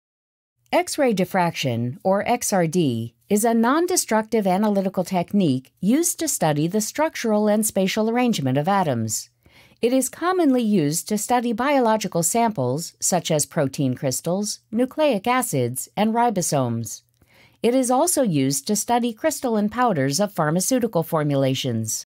X-ray Diffraction of Biological Samples | Cell Bio | Video Textbooks - Preview
X-ray diffraction or XRD is a non-destructive analytical technique used to study the structural and spatial arrangement of atoms. It is commonly used to study biological samples such as protein crystals, nucleic acids, and ribosomes. It is also used to study crystalline powders of pharmaceutical formulations.